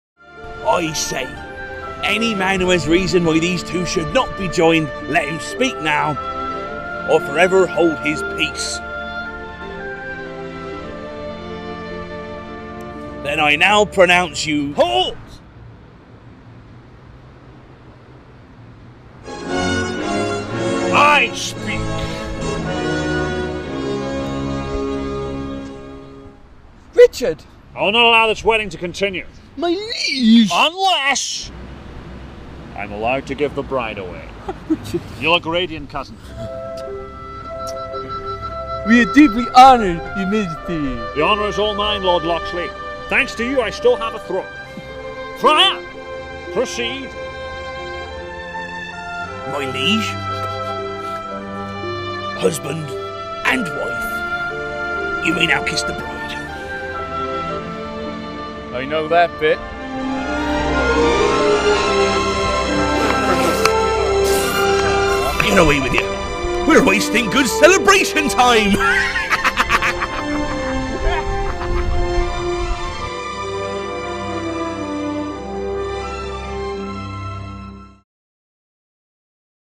PERFECT MOVIE RECREATIONS: Robin Hood Prince Of Thieves
For The Penny Dreadfuls Perfect Movie we recreated the wedding scene from Robin Hood: Prince of Thieves, which is their favourite ending to any film